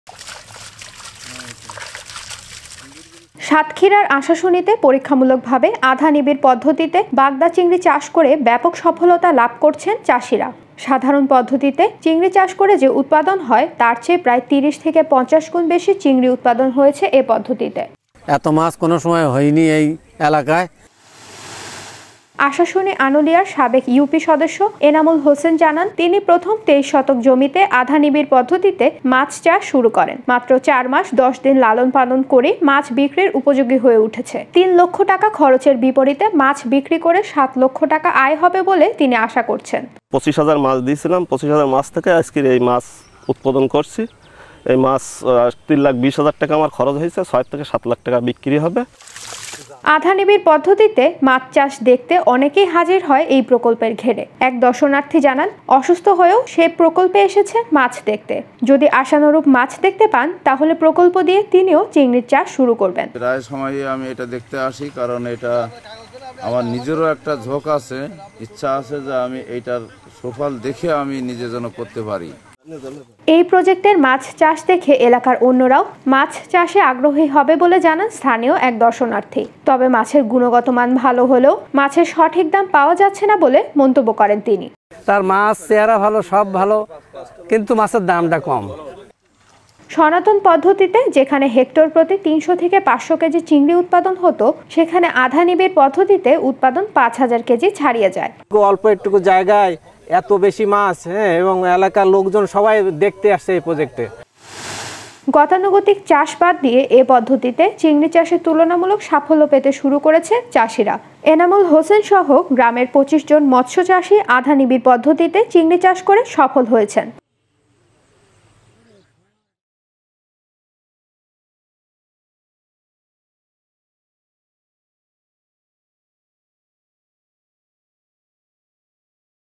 আধা নিবিড় পদ্ধতিতে বাগদা চিংড়ি চাষে ৫০ গুণ বেশি উৎপাদন | prawn
»» Office Address:
Bangladesh.